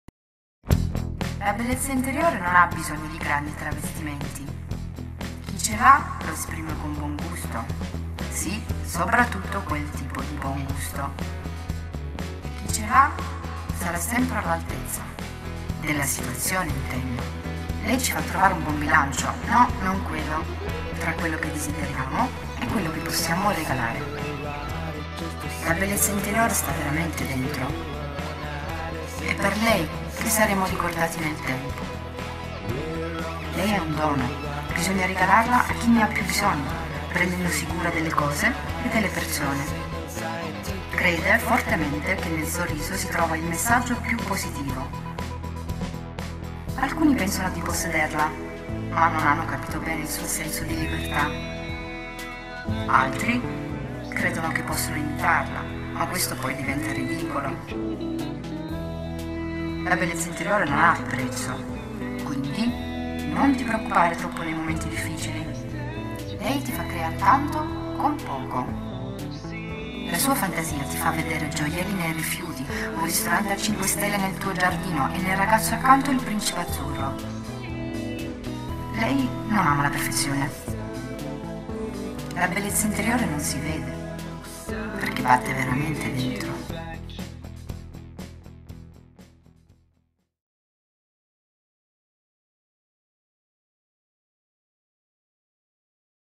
La Bellezza interiore non si vede
Inner beauty does not need great costumes...
who has it, expresses it with good taste.
Yes... especially THAT kind of good taste!
Who has will always be high up
in control of the situation I mean
It will enable us to find a good balance
NO! not that kind of balance!
Balance between what we desire
and what we can give.
Inner beauty is truly find INSIDE.
Because of our inner beauty we will be remembered.
It is a present
We must give to who need it the most.
Caring
of things and people.
It strongly believes
that a smile is the most positive message
Some think they possess it,
but they do not understand it's sense of freedom.
Others believe they can imitate it,
but then it becomes ridiculous.
Inner beauty is priceless.
So do not worry too much in difficult times.
It will enable you to create so much
with little...
Its' fantasy
will enable you to see jewelry in trash
a 5-star restaurant in your garden
and in the boy next door...
prince charming.
It doesn't like perfection.
Inner beauty is invisible.
That is why it is really to be found INSIDE...